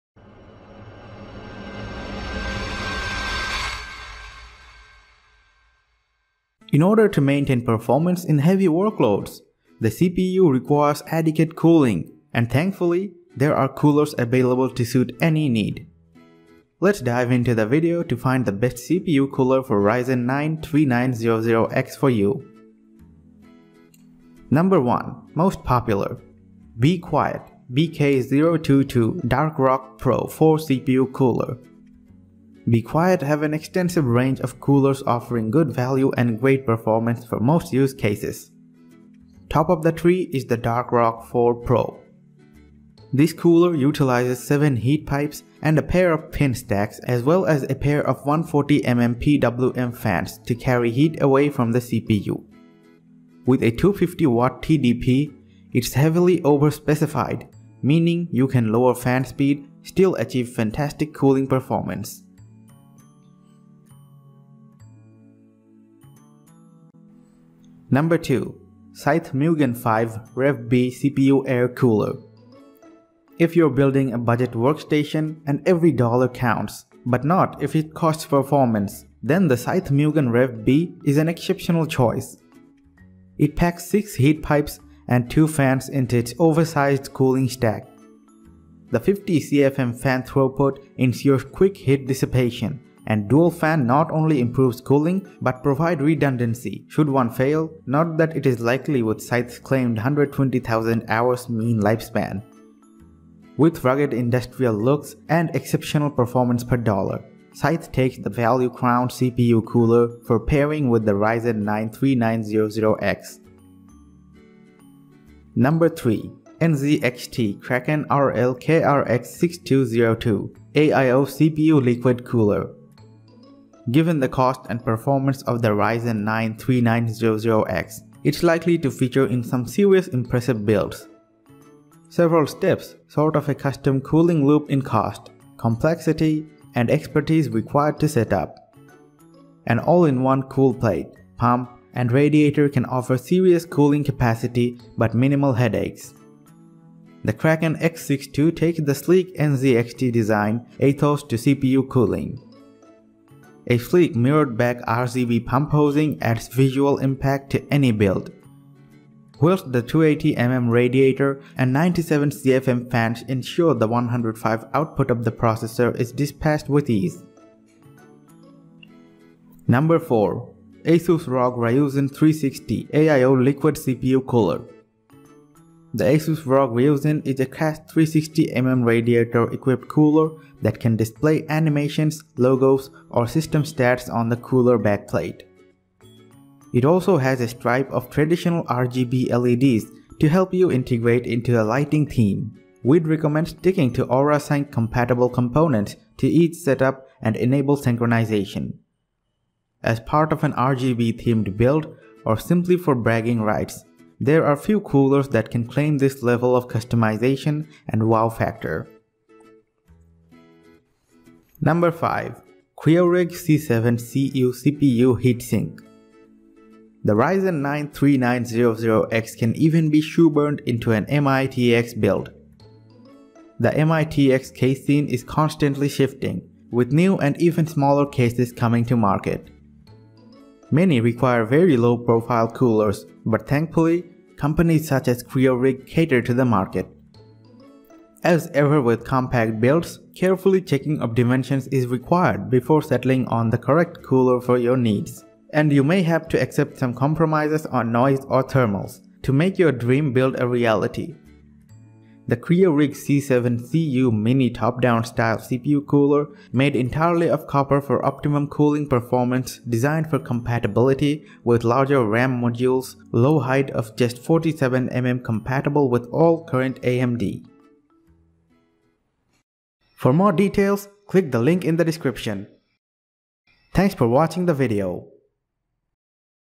Top 5 Best CPU Cooler for Ryzen 9 3900x Review in 2023
Links to the best CPU cooler for ryzen 9 3900x listed in this review video:
► 1. Be quiet! BK022 Dark Rock Pro 4 CPU Cooler
► 2. Scythe Mugen 5 Rev.B CPU Air Cooler
► 3. NZXT Kraken RL-KRX62-02 AIO CPU Liquid Cooler
► 4. ASUS Rog Ryujin 360 AIO Liquid CPU Cooler
► 5. CRYORIG C7 Cu CPU Heatsink

🕝Timestamps🕝
0:07 - Introduction
0:24 - Be quiet! BK022 Dark Rock Pro 4 CPU Cooler
1:08 - Scythe Mugen 5 Rev.B CPU Air Cooler
1:55 - NZXT Kraken RL-KRX62-02 AIO CPU Liquid Cooler
2:49 - ASUS Rog Ryujin 360 AIO Liquid CPU Cooler
3:31 - CRYORIG C7 Cu CPU Heatsink

★What is a CPU cooler for ryzen 9 3900x?
Computer cooling is required to remove the waste heat produced by computer components, to keep components within permissible operating temperature limits.